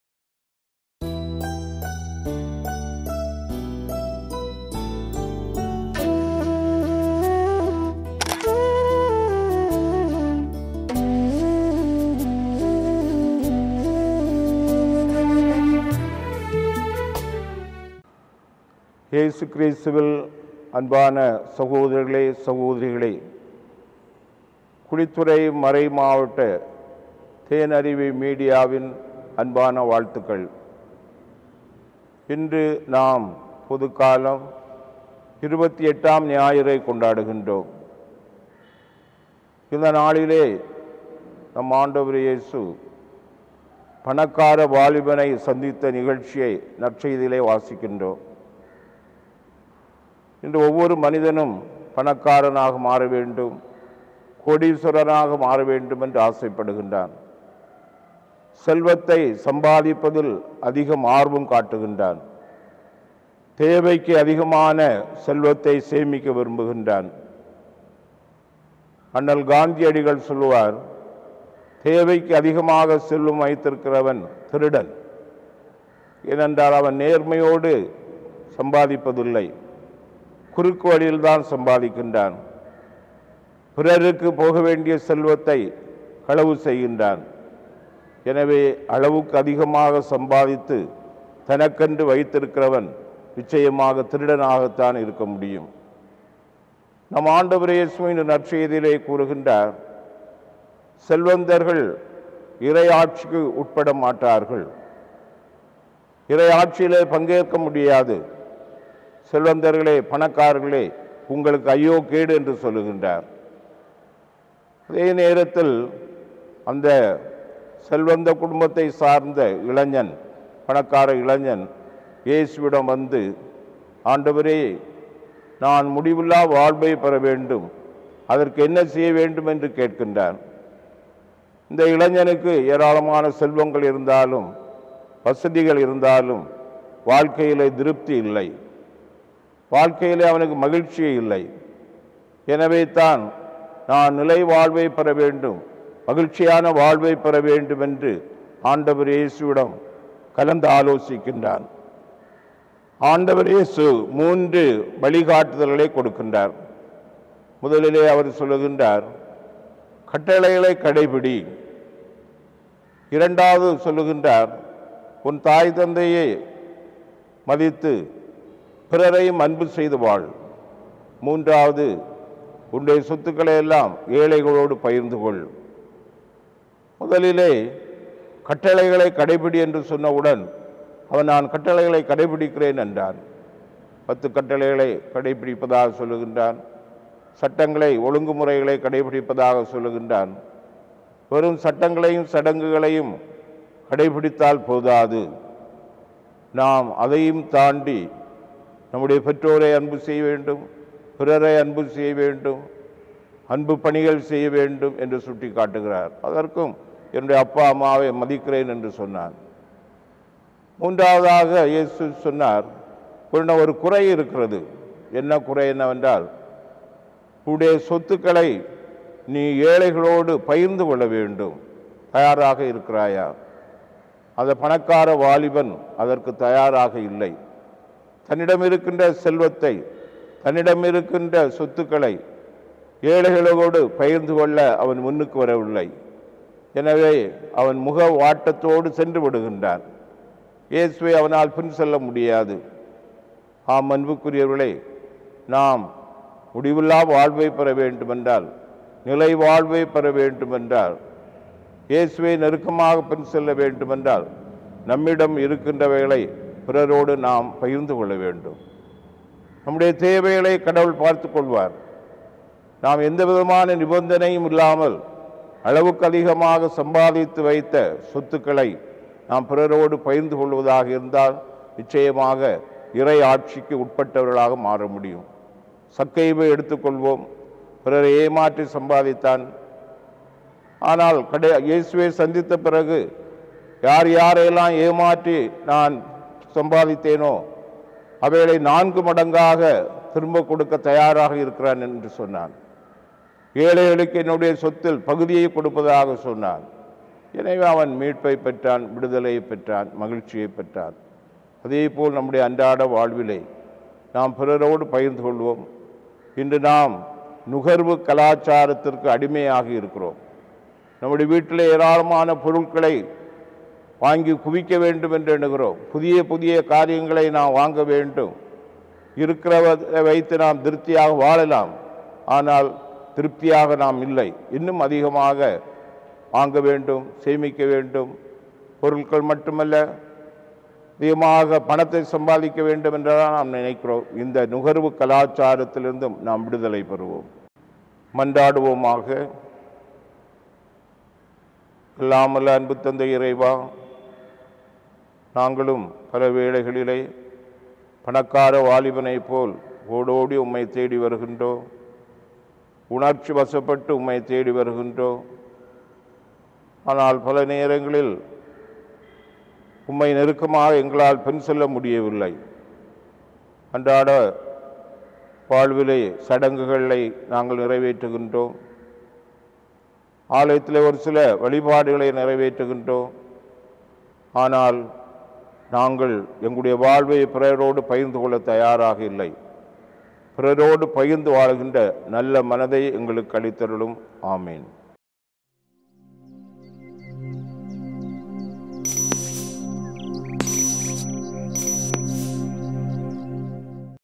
இன்றைய இறை சிந்தனை |Daily Gospel Reflection by Rev. Fr Jesu Rethinam | 13/10/2024 |Tamil Sermon
இன்று நமக்காக இறை சிந்தனைகளை வழங்குபவர் அருட்பணி. இயேசு ரெத்தினம் , பங்குத்தந்தை, புனித மிக்கேல் அதிதூதர் ஆலயம் புலியூர்குறிச்சி

 ஒருங்கிணைப்பு : அருட்பணி. சுனில், செயலர், சமூகத் தொடர்பு பணிக்குழு, குழித்துறை மறைமாவட்டம்.

Camera & Editing :  A. Augustin